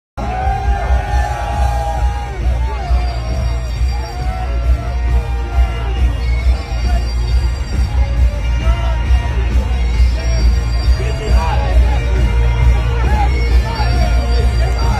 James Hetfield smoking with fans
James Hetfield lighting fans cigars